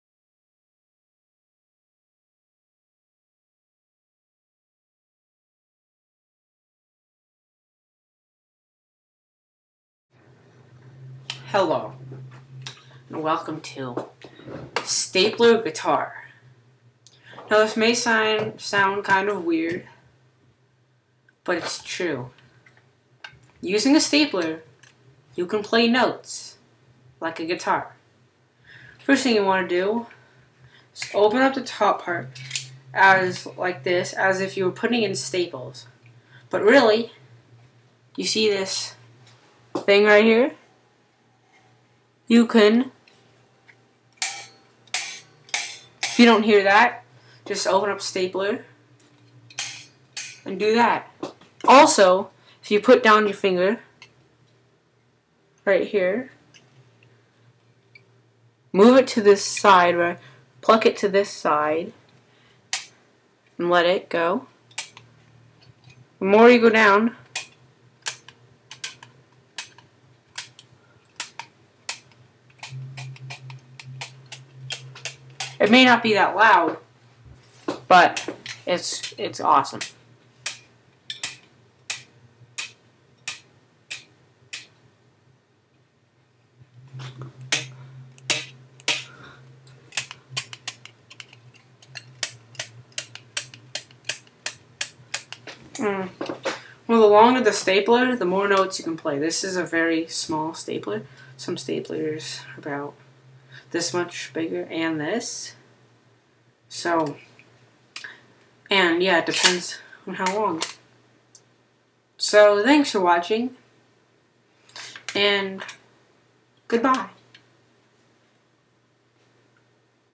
a stapler guitar
this video will teach you how to make a stapler guitar